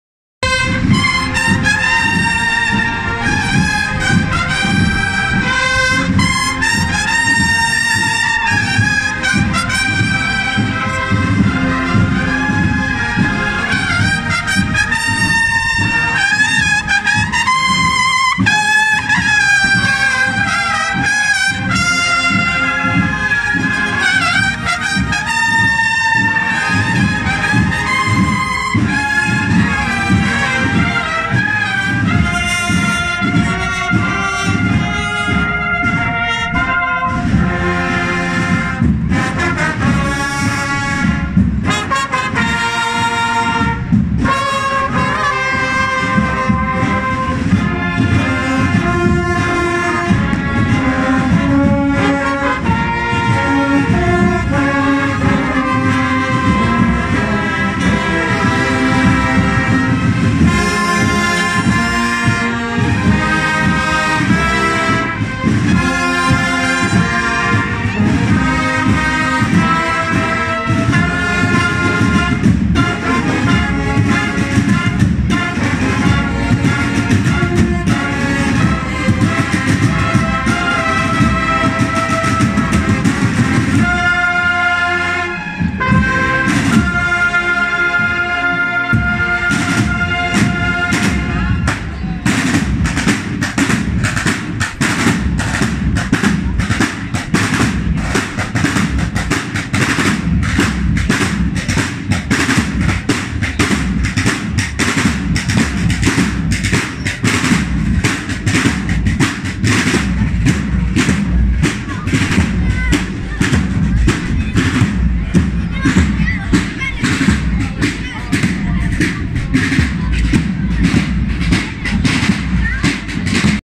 Bcctt. N. S. DEL SOL ☀ SEVILLA. En la cruz del serrillo de Villalba del halcor Huelva.
Magnífico repertorio musical 🎶 por las calles de Villalba del halcor Huelva 2.6.2019.aop